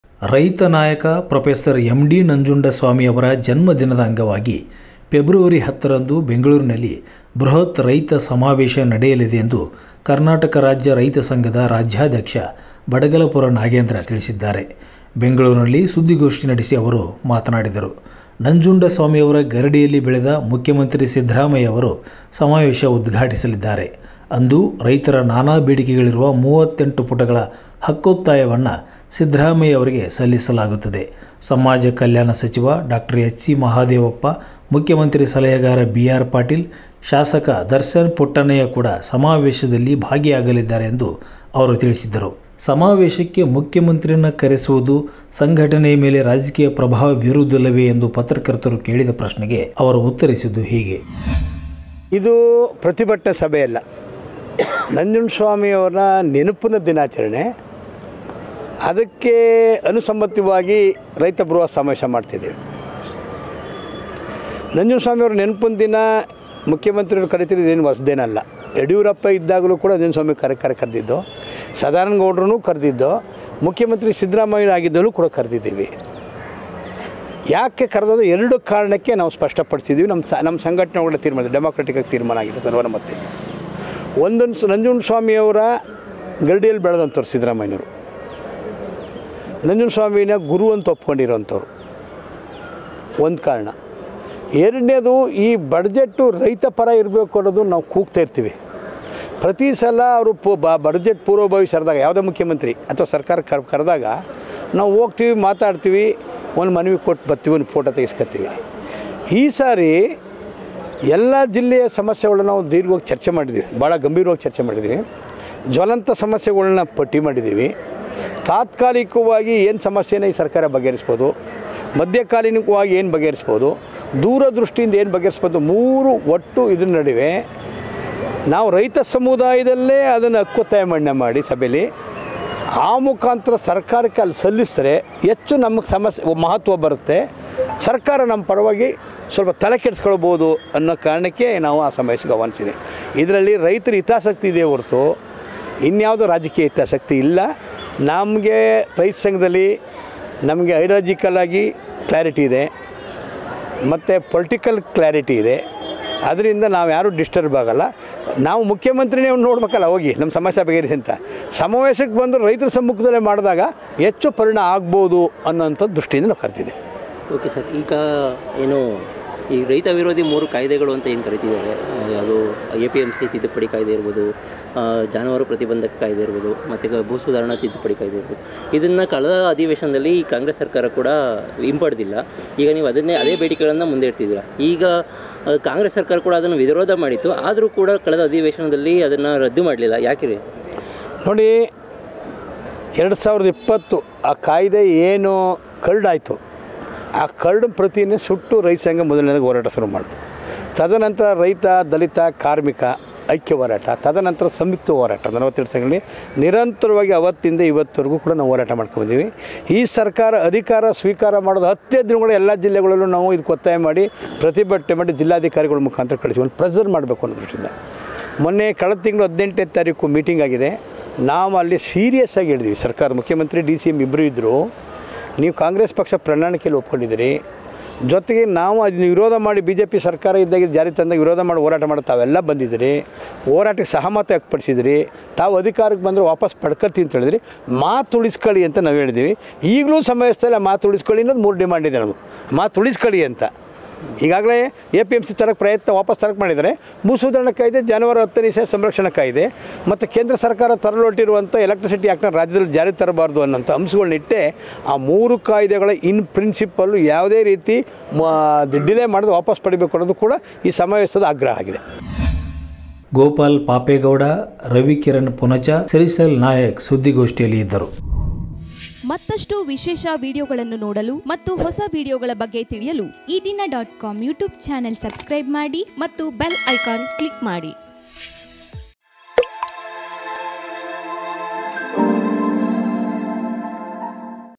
ರೈತ ವಿರೋಧಿ ಕಾನೂನು ವಾಪಸ್ ಪಡೆಯಲು ಸಿಎಂಗೆ ಹಕ್ಕೊತ್ತಾಯ ಸಲ್ಲಿಸುತ್ತೇವೆ | ಬಡಗಲಪುರ ನಾಗೇಂದ್ರ
ರೈತ ನಾಯಕ ಪ್ರೋ.ಎಂ.ಡಿ ನಂಜುಂಡಸ್ವಾಮಿ ಅವರ ಜನ್ಮದಿನದ ಅಂಗವಾಗಿ ಫೆಬ್ರವರಿ 10ರಂದು ಬೆಂಗಳೂರಿನಲ್ಲಿ ಬೃಹತ್ ರೈತ ಸಮಾವೇಶ ನಡೆಯಲಿದೆ ಎಂದು ಕರ್ನಾಟಕ ರಾಜ್ಯ ರೈತ ಸಂಘದ ರಾಜ್ಯಾಧ್ಯಕ್ಷ ಬಡಗಲಪುರ ನಾಗೇಂದ್ರ ತಿಳಿಸಿದ್ದಾರೆ.